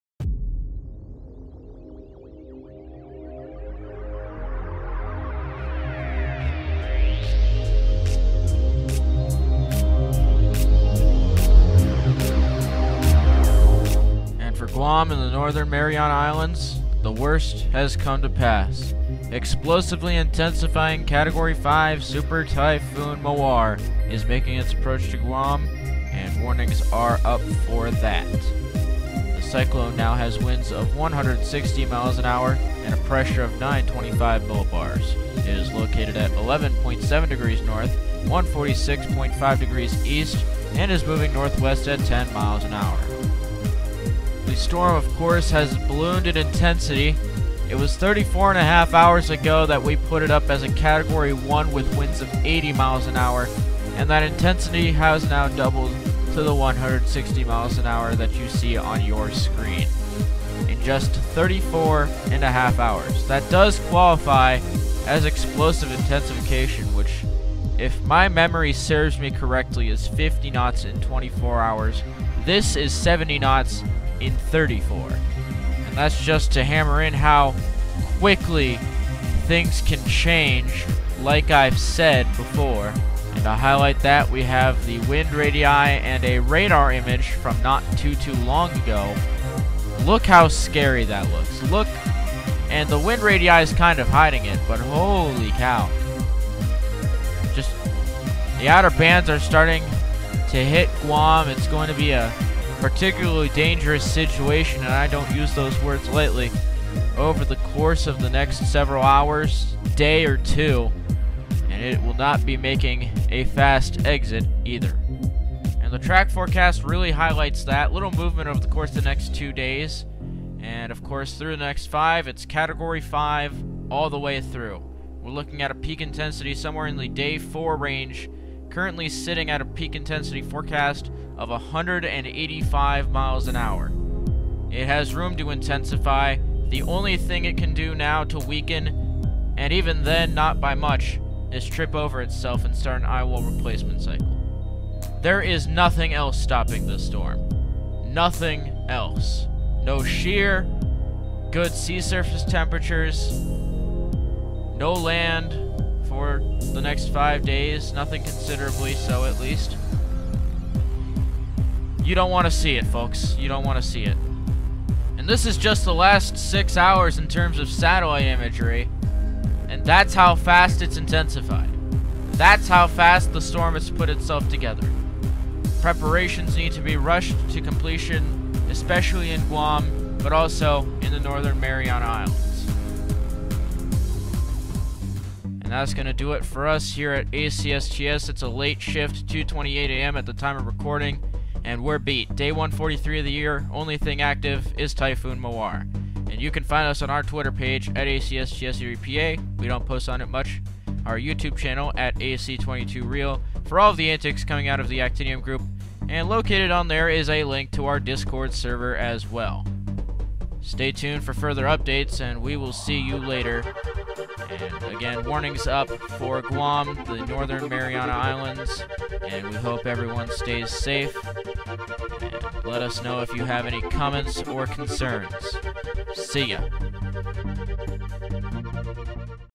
[STS] Mawar unexpectedly and explosively intensifies to Category 5 status; to strike Guam + environs
A miracle would surely be appreciated right about now.

date should say may 23 not 21